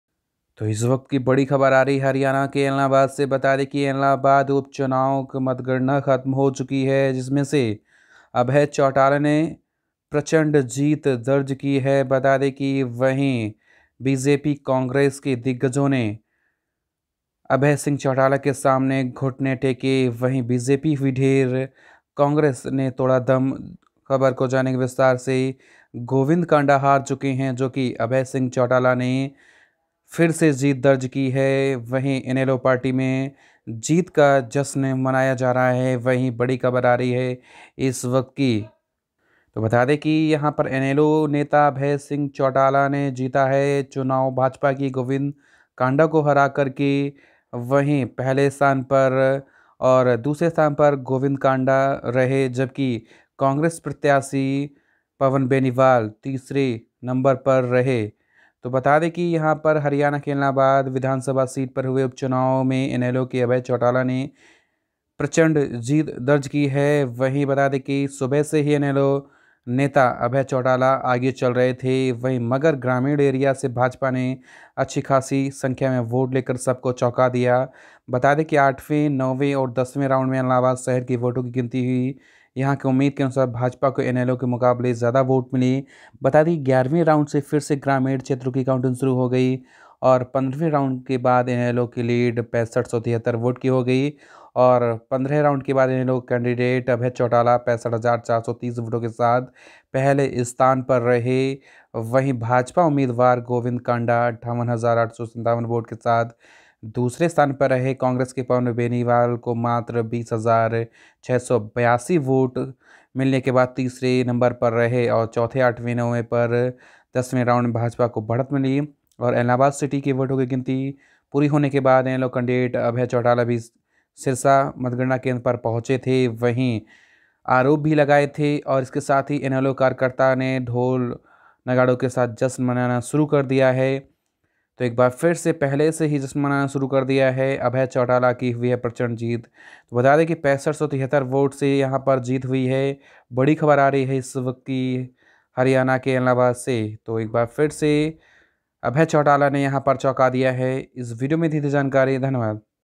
ऐलनाबाद उपचुनाव अभय चौटाला ने ऐतिहासिक 6573 वोटों से जीत की दर्ज | भाजपा कांग्रेस का सूपड़ा साफ
ellenabad election result,ellenabad vote counting,ellenabad result today,abhay singh chautala win, congress losses,sirsa up chunav result,haryana news,the knowledge guru,